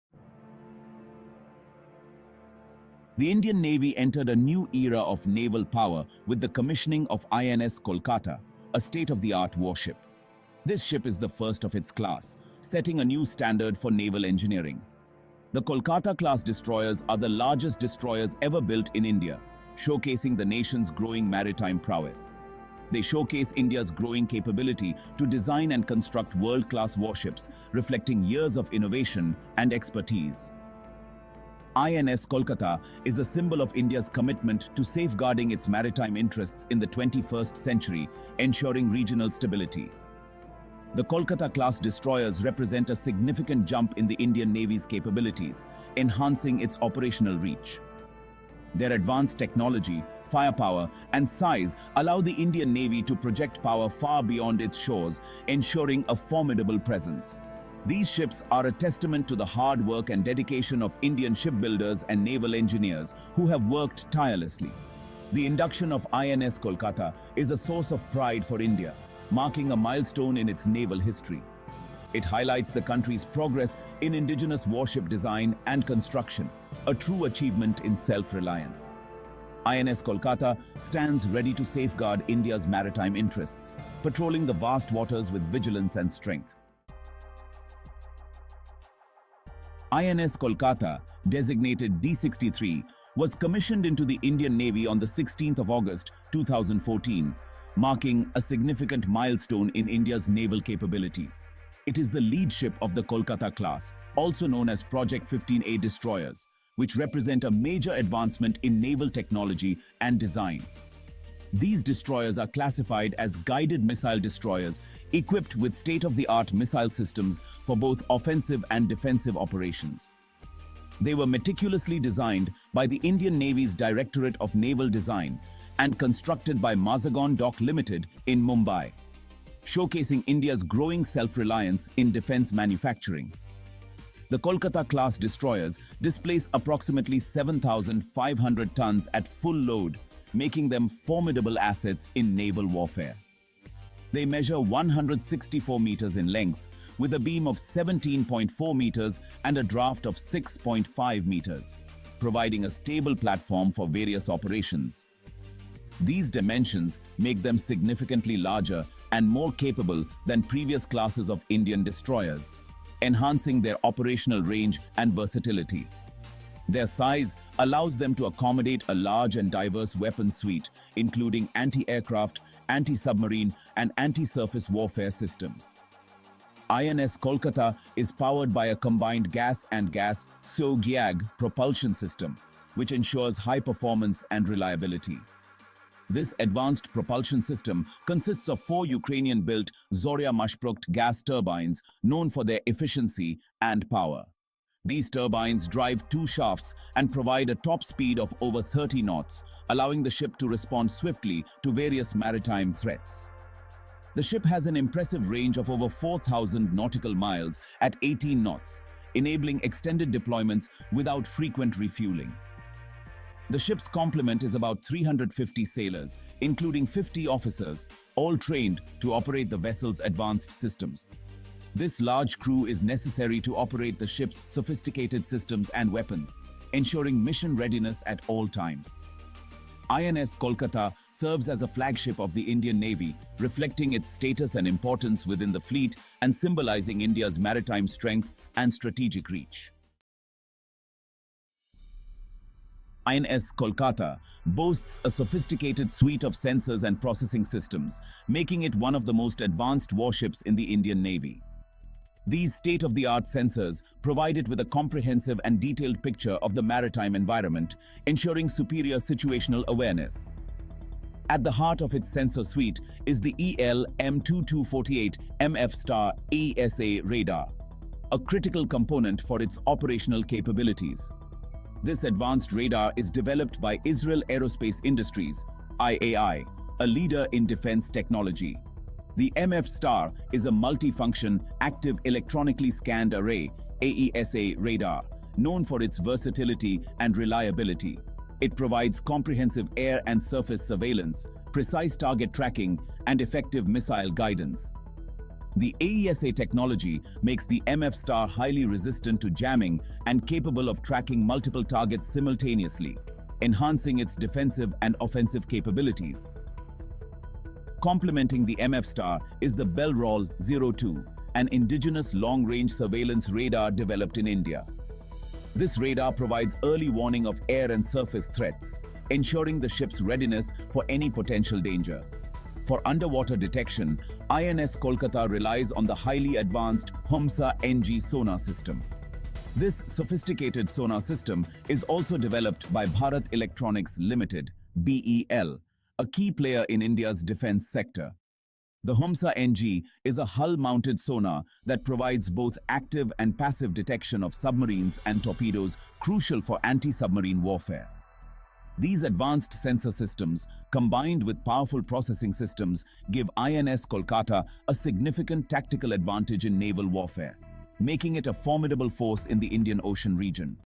🇮🇳 INS Kolkata: India's DEADLIEST Destroyer 💣 | Defense in Short India
Dive deep with Defense in Short India as we explore the INS Kolkata, the lead ship of the Kolkata-class stealth guided-missile destroyers and a cornerstone of the Indian Navy's might! ⚓

In this video, we'll uncover:

Cutting-edge weaponry: BrahMos missiles, Barak 8 SAMs, and more!
Stealth capabilities: How the INS Kolkata evades enemy radar
Advanced technology: A look at the ship's impressive sensors and combat systems
Role in the Indian Navy: Protecting India's maritime interests
Don't miss out on this exciting exploration of India's naval power!